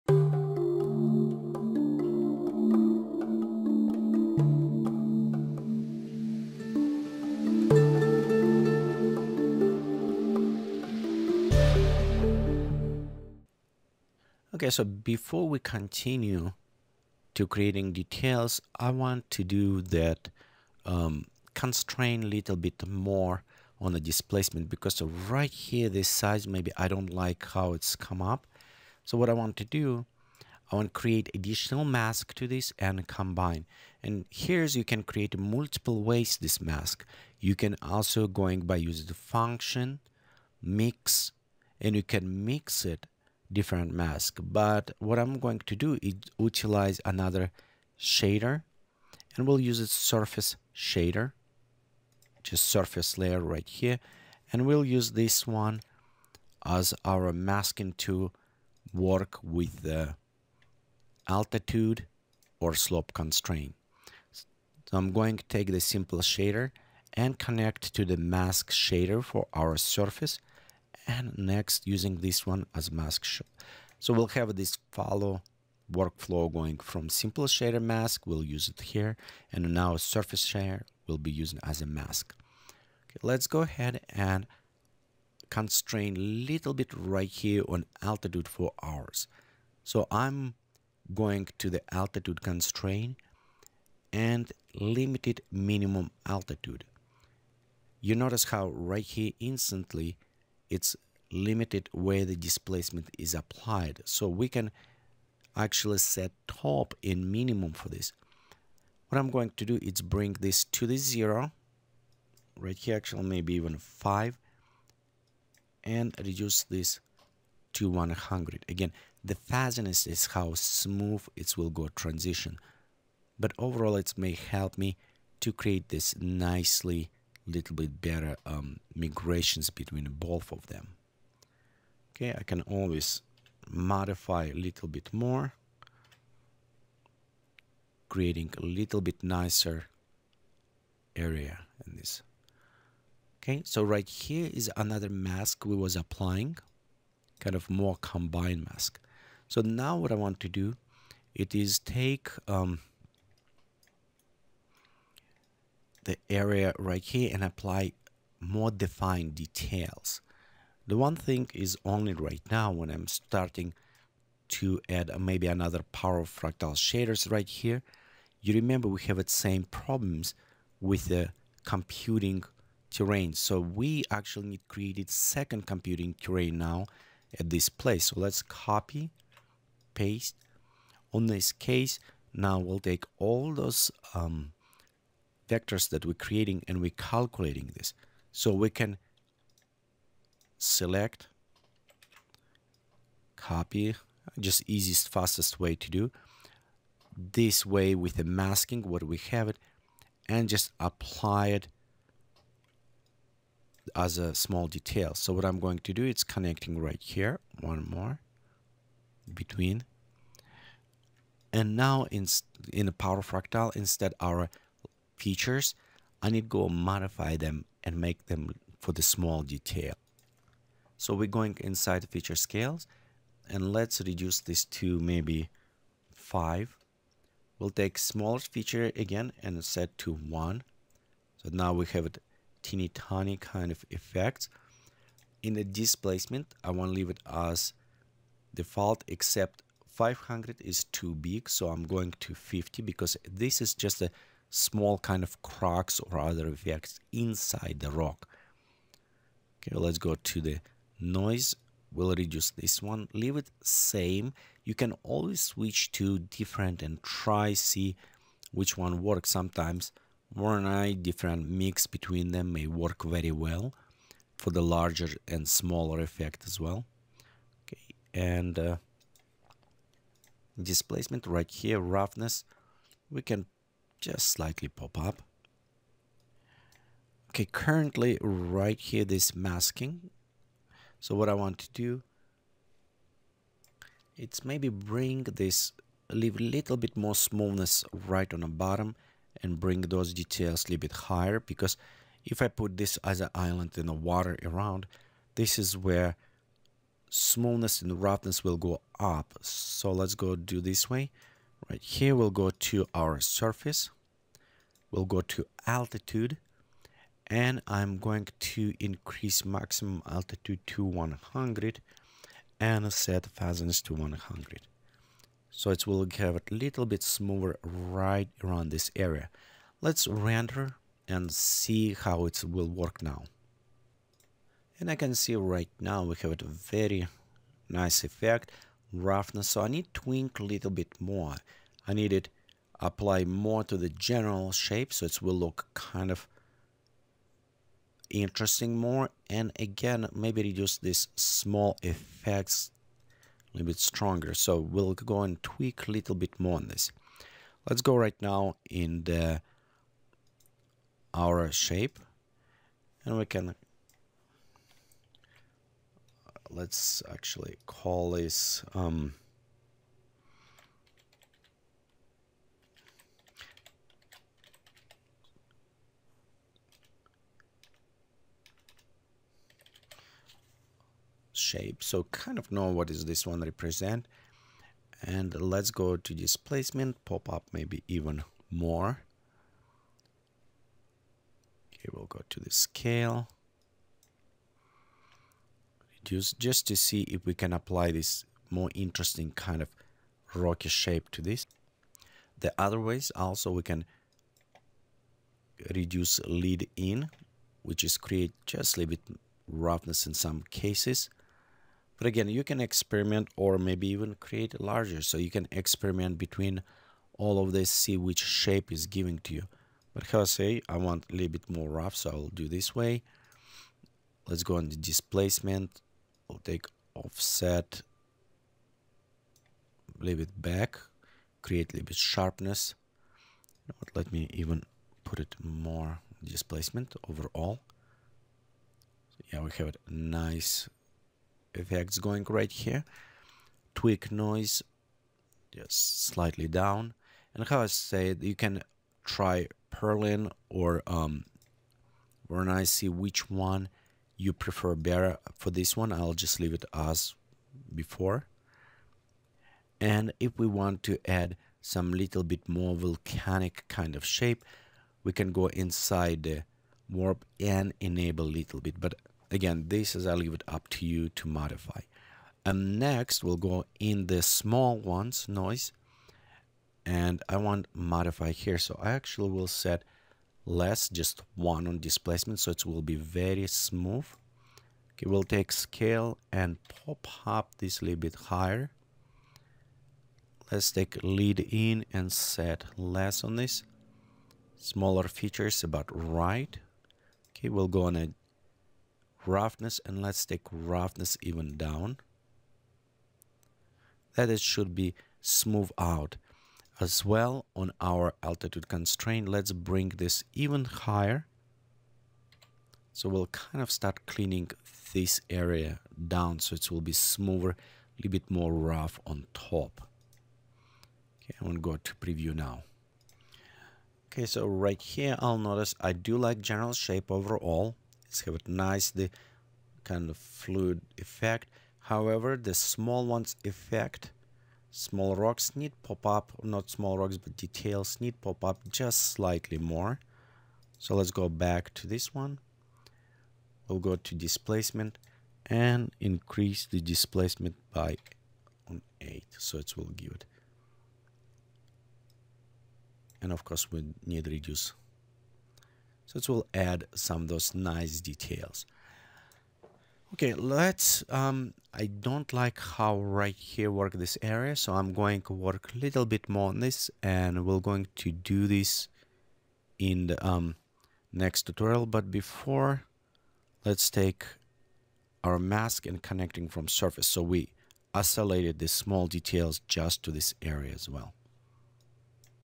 Rock Island Details displacement 3
Exploring Terragen 3, series one.
Project based tutorials.
You can purchase this and more tutorials, including bonus packs